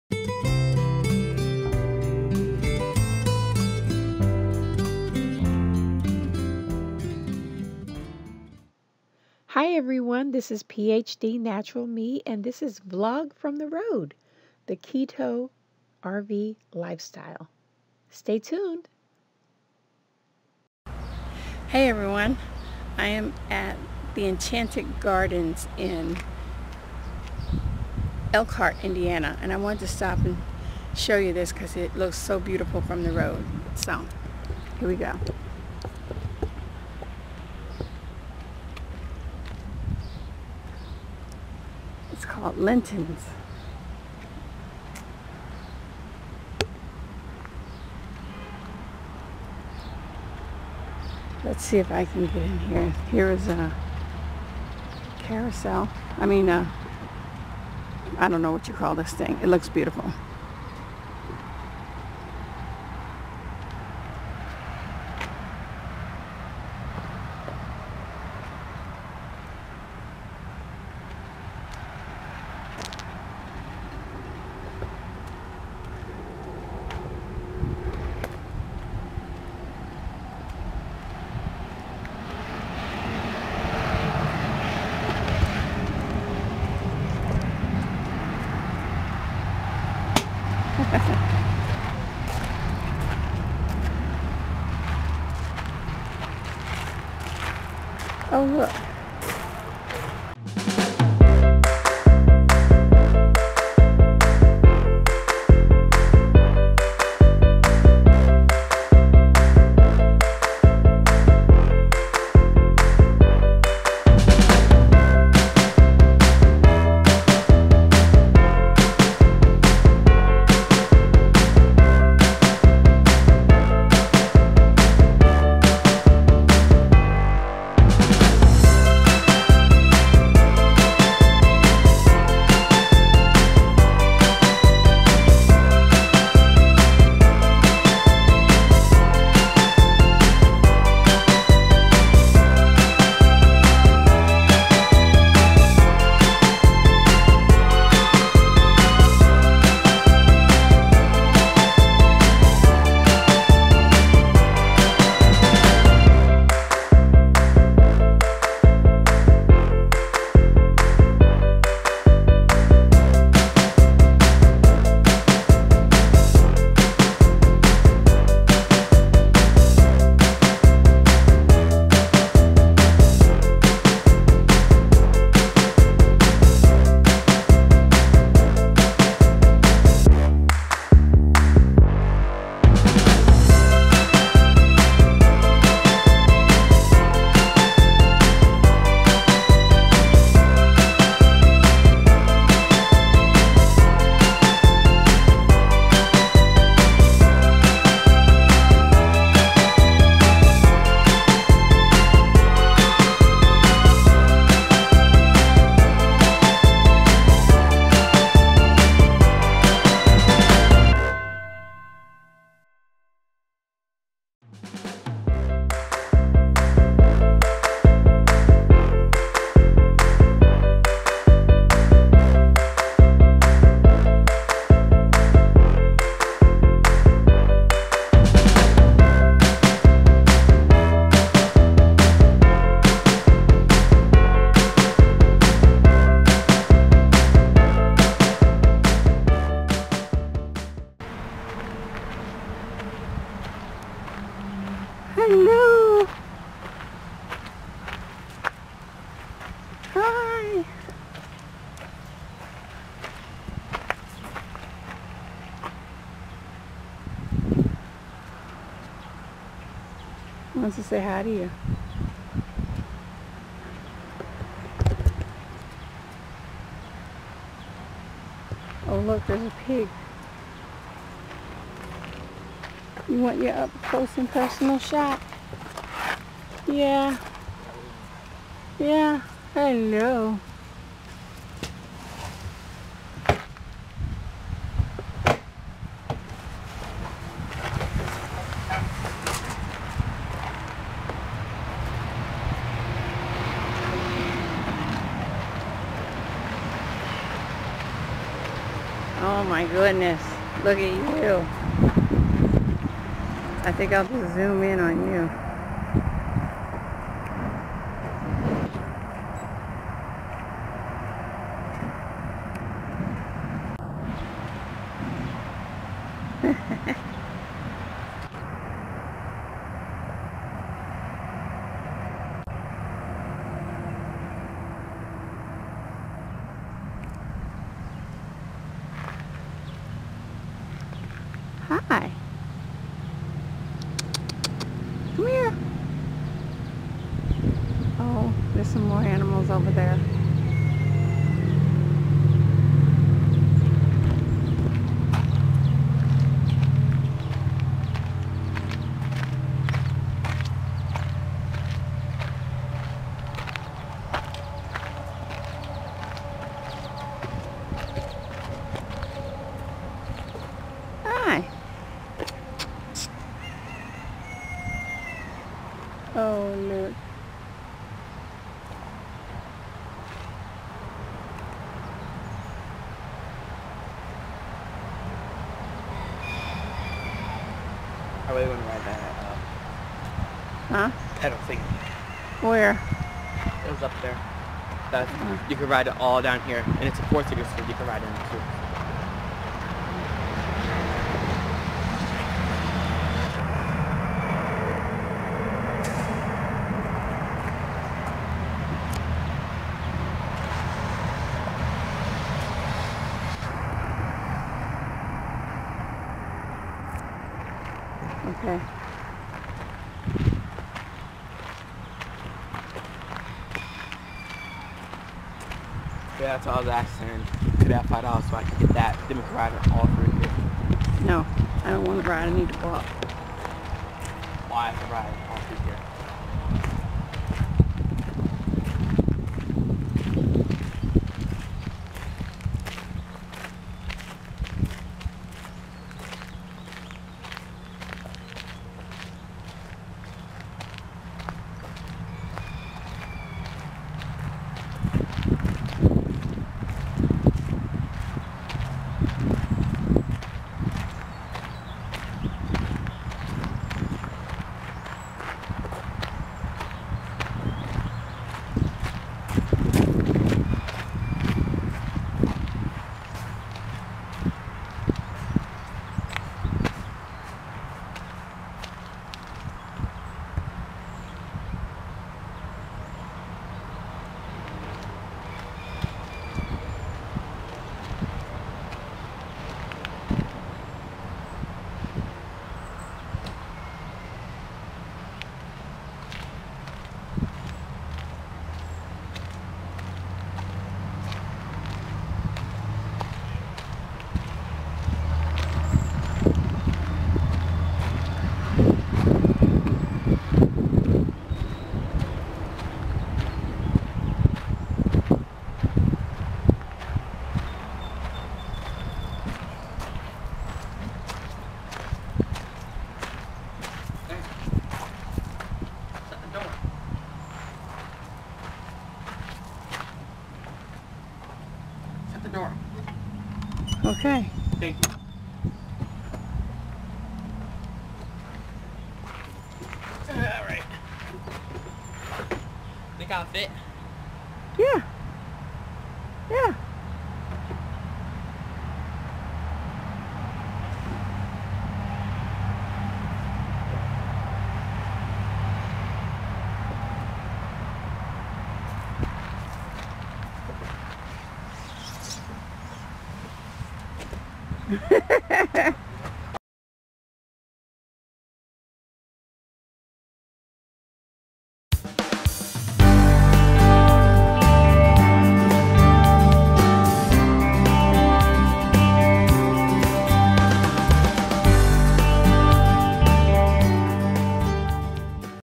Enchanted Garden Shop in Elkhart, Indiana | RV Lifestyle
Take a look at this enchanted garden center in Elkhart, Indiana

Stay Healthy!